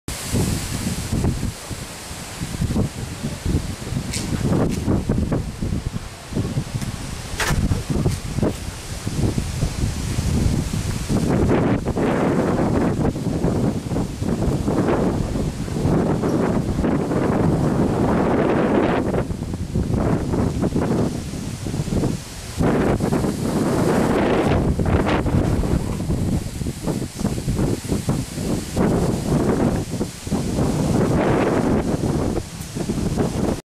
DOLAZAK OLUJE NEVREME 2 8 2020
Približavanje nevremena sa neverovatnim oblacima iznad SUBOTICE kao da izviru iz  jedne tačke